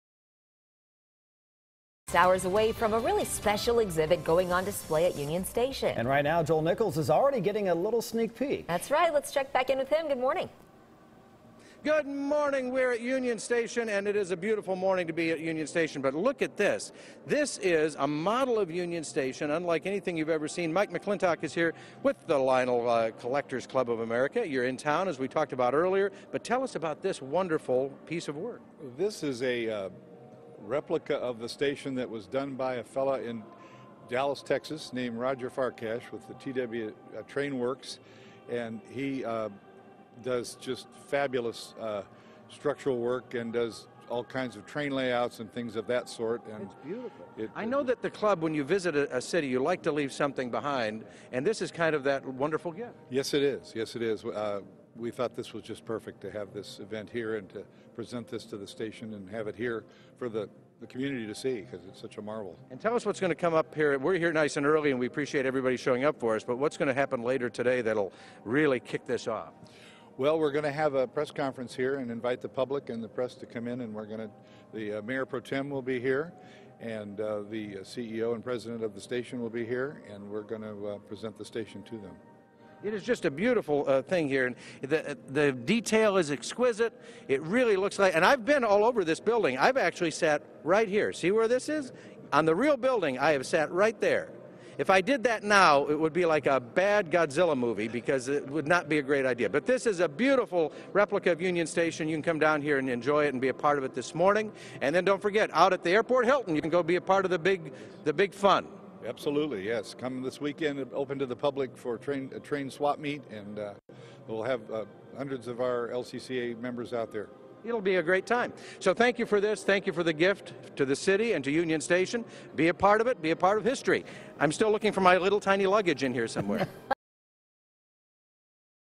Special exhibit to be unveiled at Union Station
The Lionel Collectors Club of America is having its convention in Kansas City. The group is giving Union Station a replica of the station.

41 Action News, KSHB, brings you the latest news, weather and investigative reports from both sides of the state line.